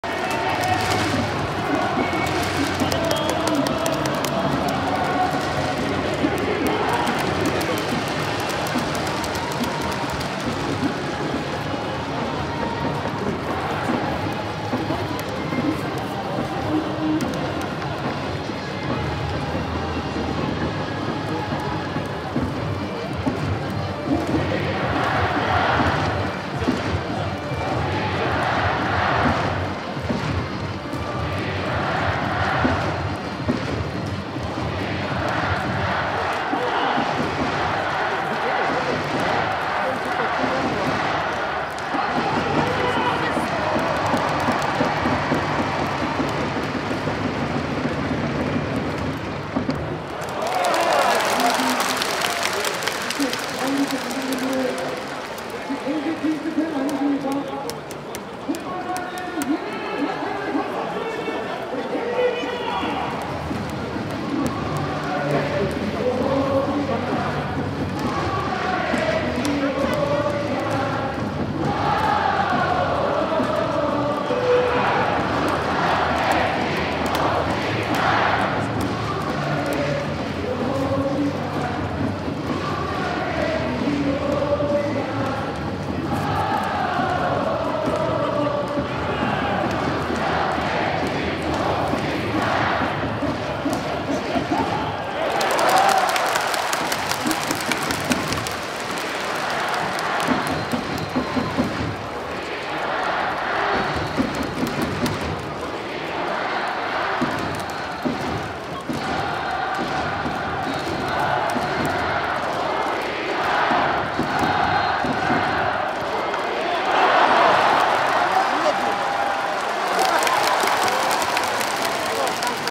2022.04.30 (VS 롯데 2차전) 2회말 오지환 타석 (3루 외야 시점) 4K 직캠!
3루 외야에서 찍은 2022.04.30 (VS 롯데 2차전) 2회말 오지환 선수 타석 직캠 영상 입니다!